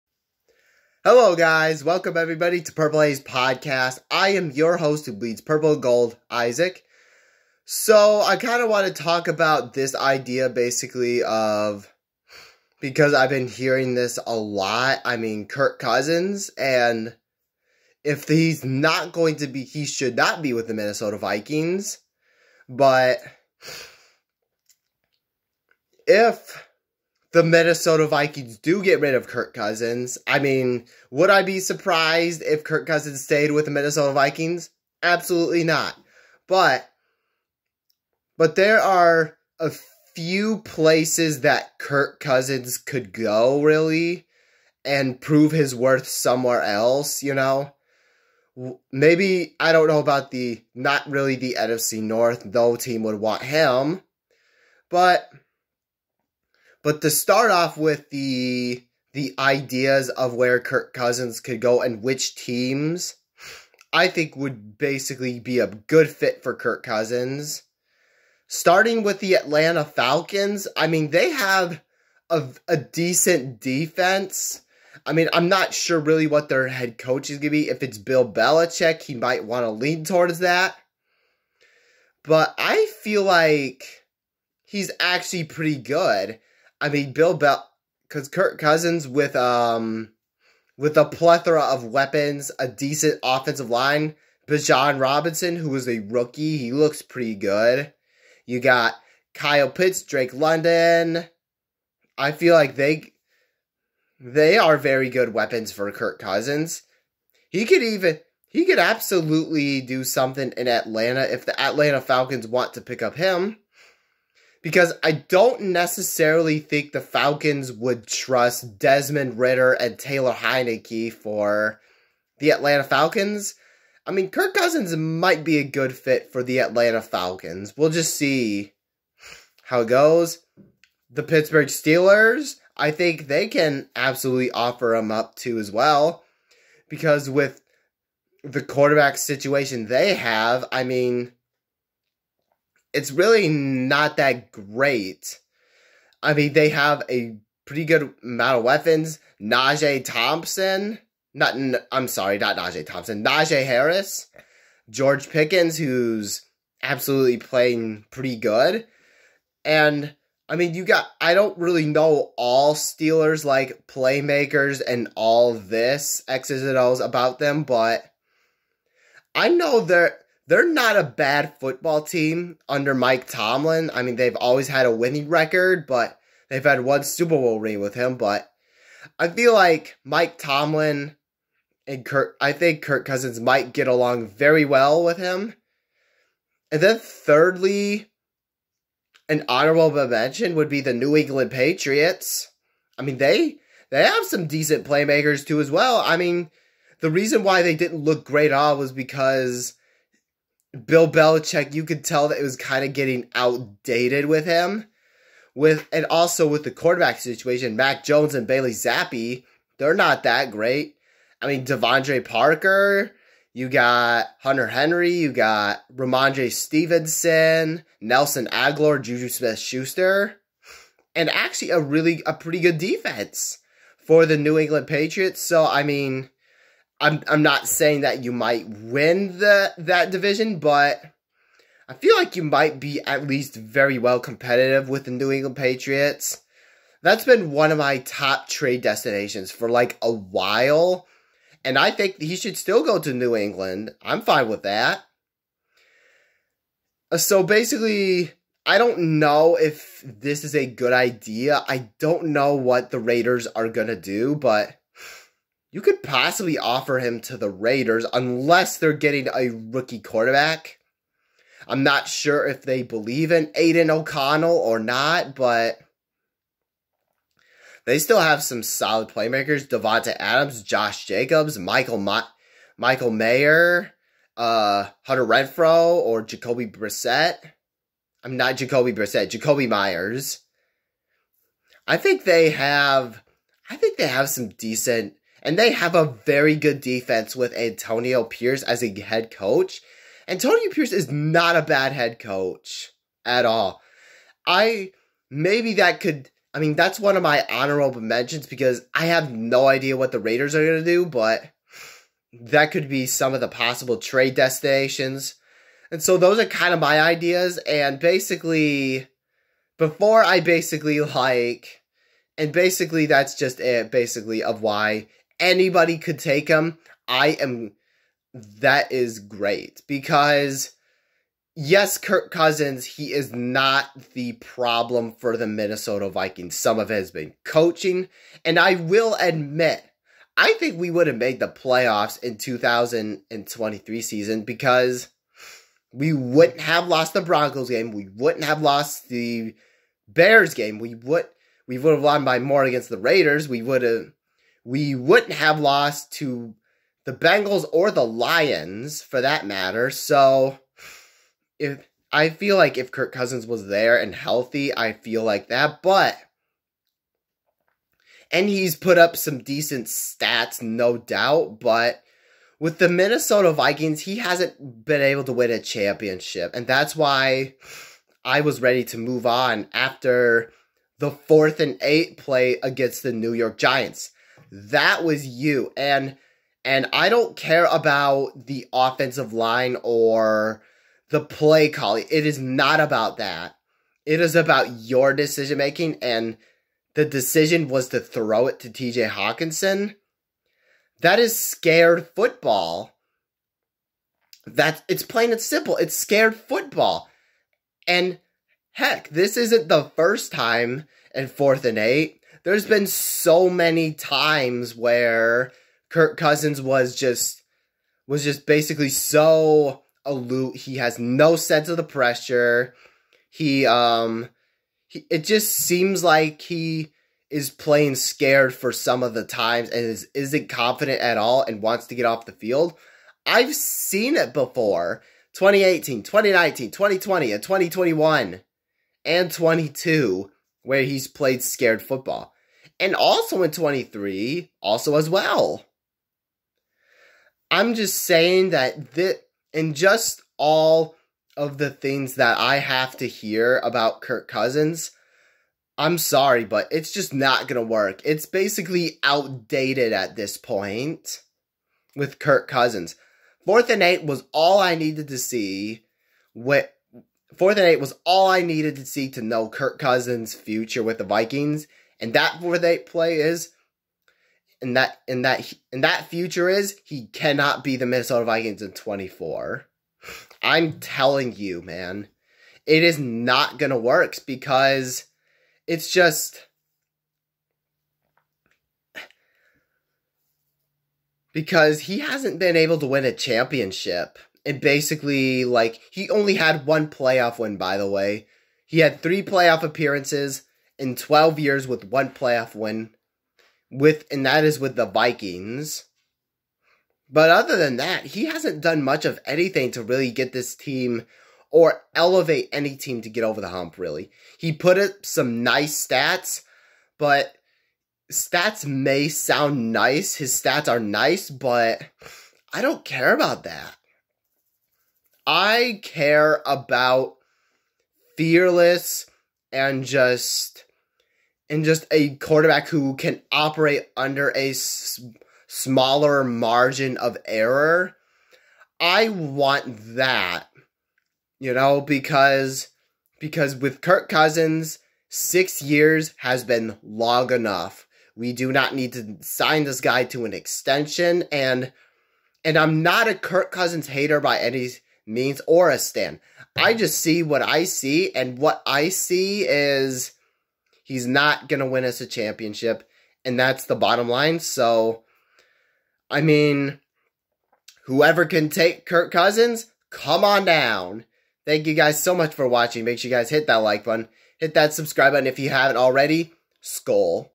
Kirk Cousins FA or Trade Destinations
We dive into a few teams that can fit well with Kirk Cousins and why the Cousins hasn't won us a championship. I finally got batteries to turn my Vikings light on LOL